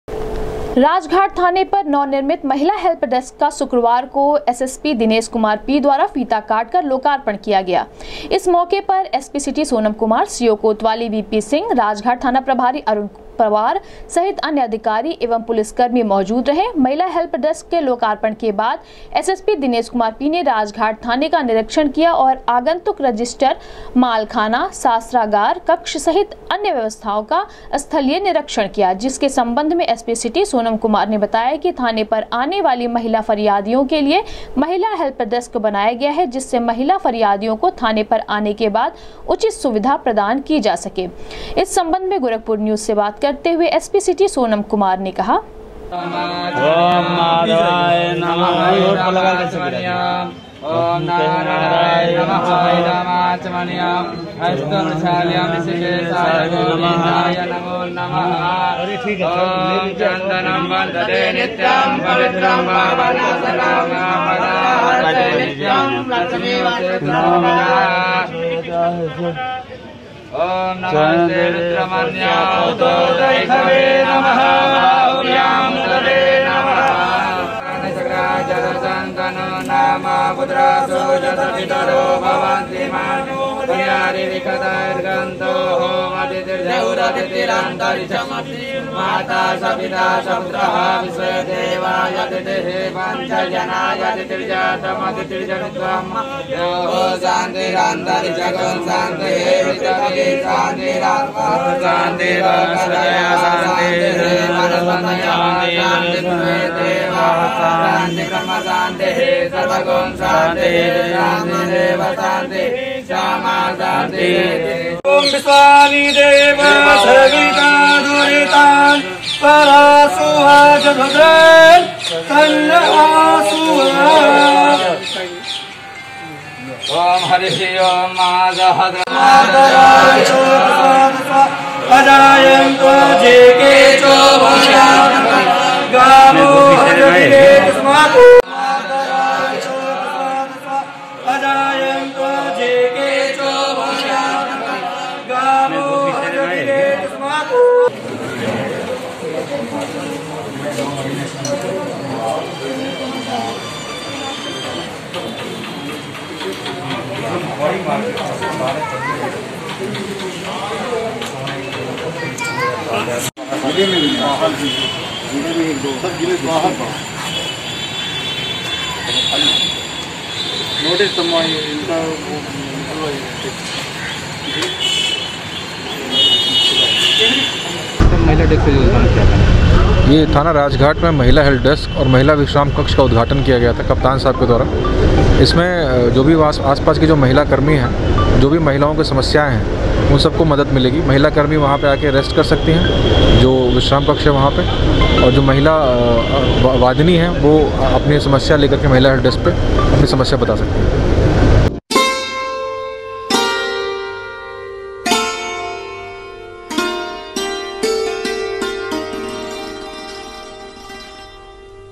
राजघाट थाने पर नवनिर्मित महिला हेल्प डेस्क का शुक्रवार को एसएसपी दिनेश कुमार पी द्वारा फीता काटकर
राजघाट थाने पर नवनिर्मित महिला हेल्प डेस्क का शुक्रवार को एसएसपी दिनेश कुमार पी द्वारा फीता काटकर किया गया लोकार्पण,थाने का किया निरीक्षण, एसपी सिटी ने कहा,महिला हेल्प डेस्क से थाने पर आने वाली महिला फरियादियो को मिलेगी राहत